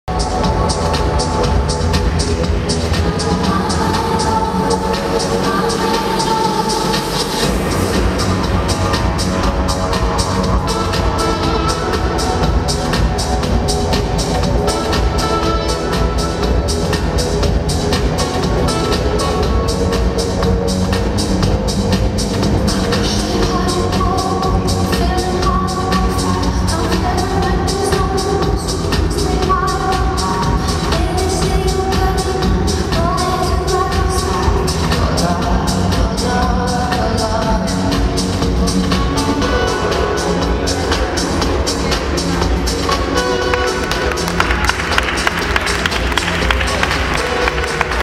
Xiao Yun Lin / Szaju design divatbemutatójának fináléja 2016 S/S Cuki
Budapesti Tavaszi Fesztivál 2016 április
Nemzeti Múzeum